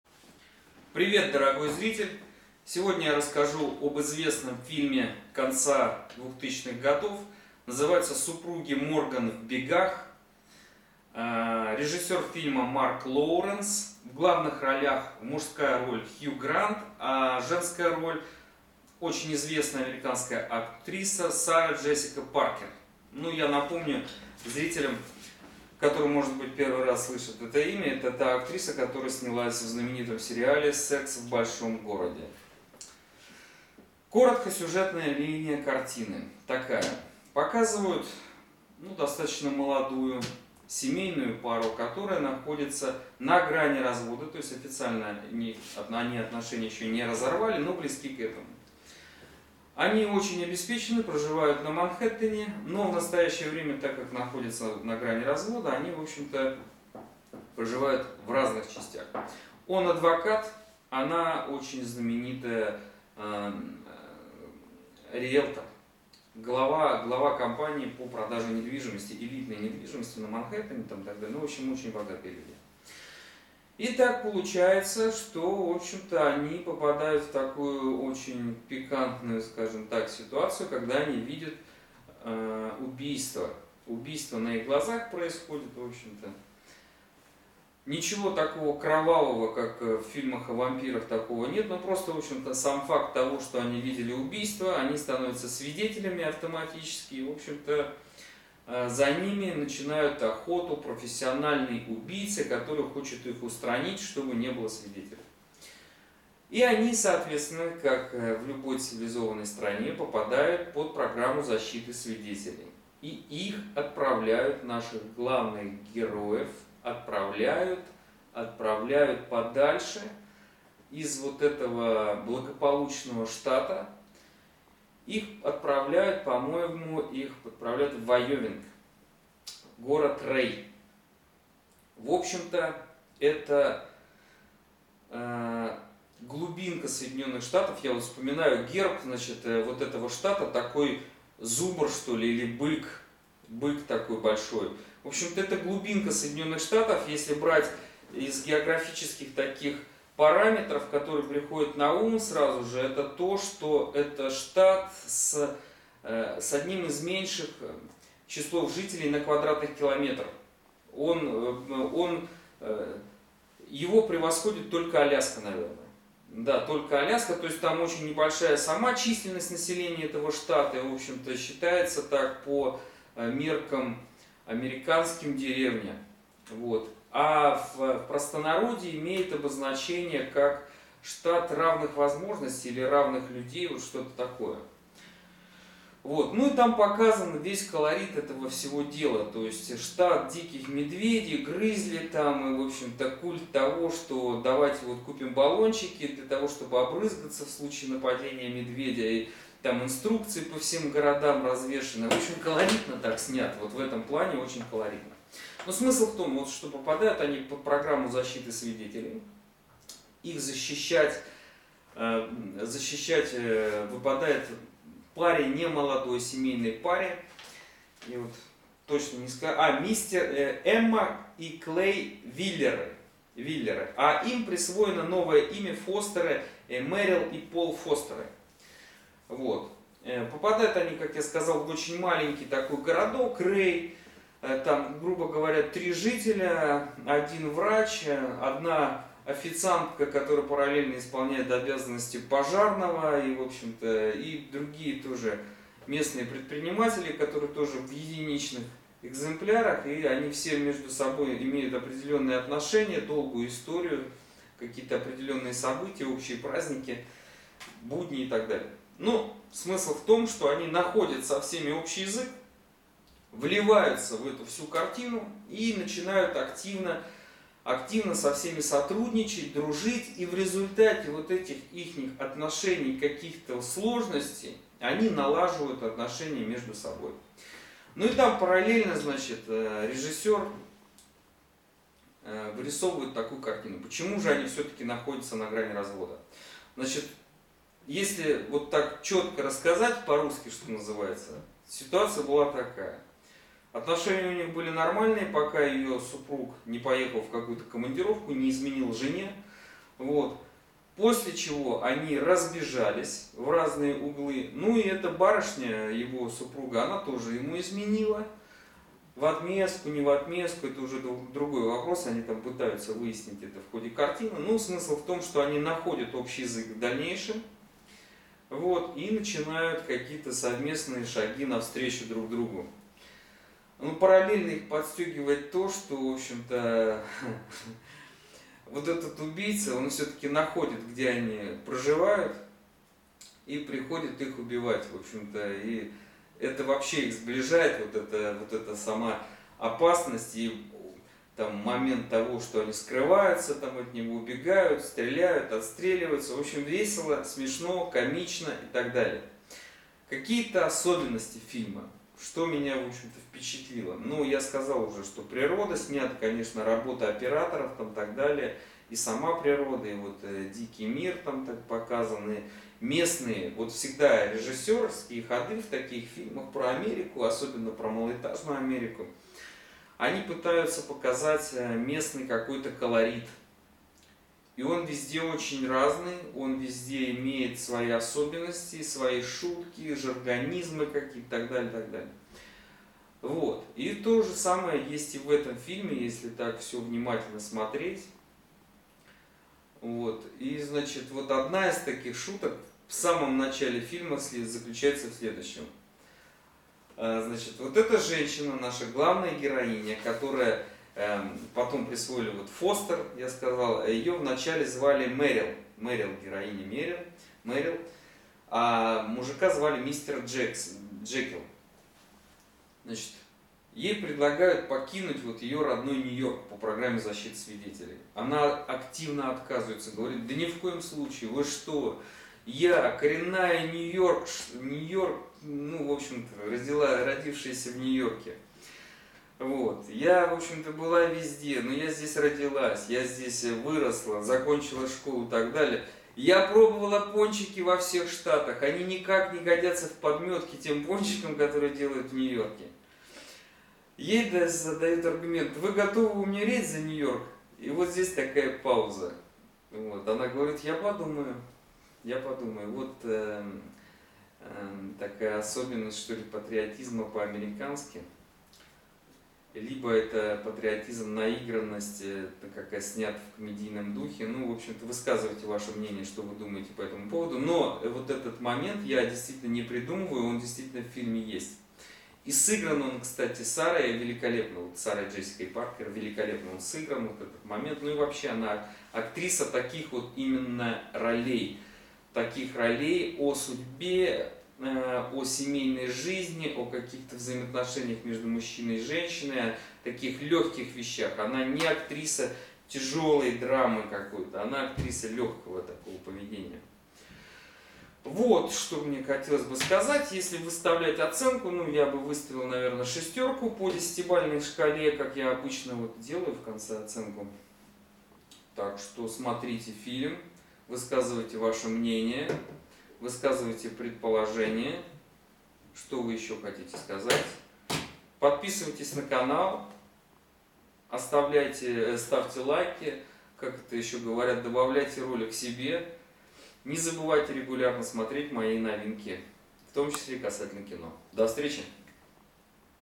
О фильме "Супруги Морган в бегах" , режиссёр Марк Лоуренс
Морганы - типичная современная Нью-Йоркская чета с Манхеттена. Они богаты, успешны, циничны, избалованы комфортом большого города и ещё... они на грани развода. Как нельзя более вовремя они становятся случайными очевидцами убийства и теперь вынуждены уехать в небольшой провинциальный городок, по программе защиты свидетелей. Но вместе с потерей привычных городских прелестей жизни они вновь обретают интерес друг к другу..